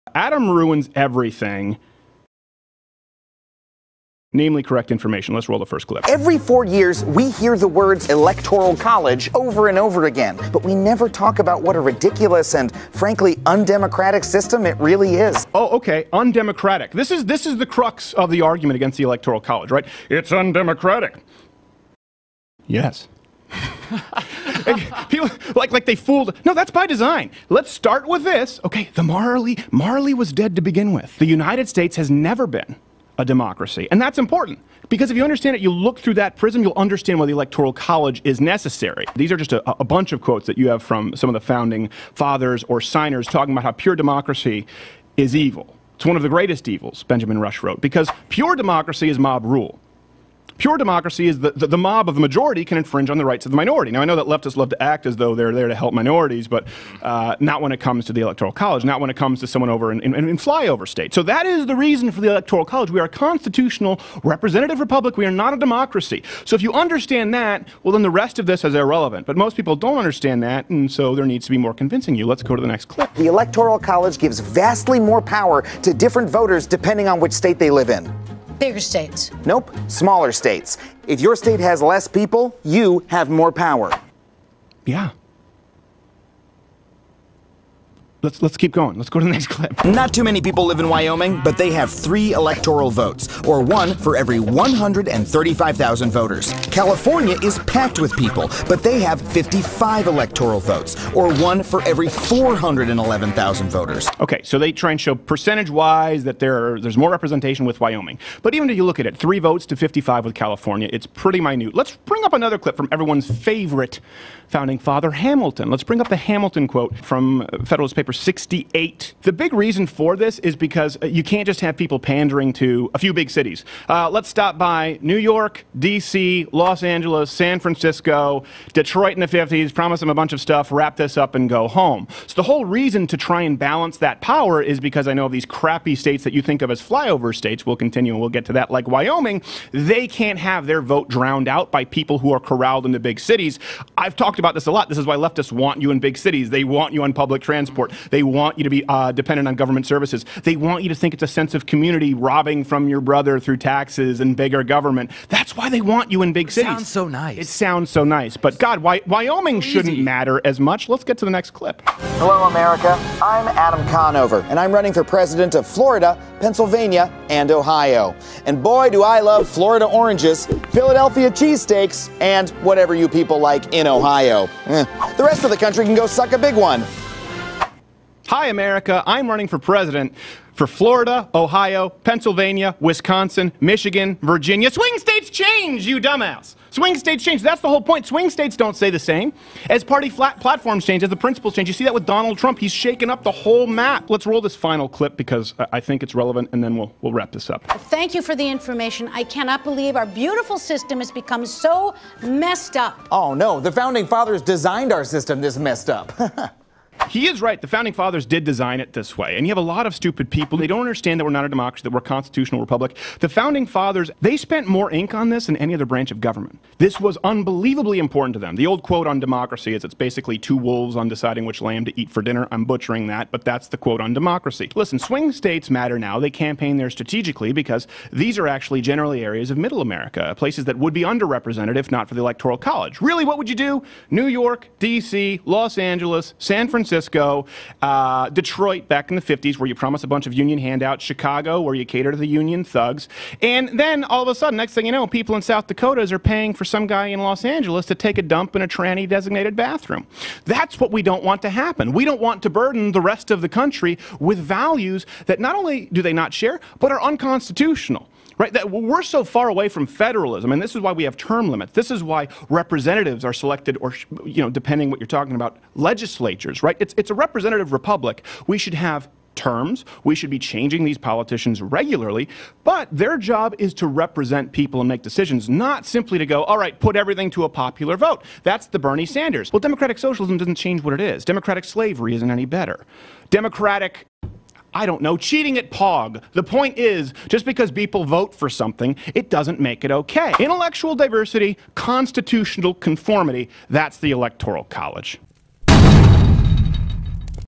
Adam Ruins Correct Information: The Electoral College | Crowder Classics
Steven debunks another terrible Adam Conover video.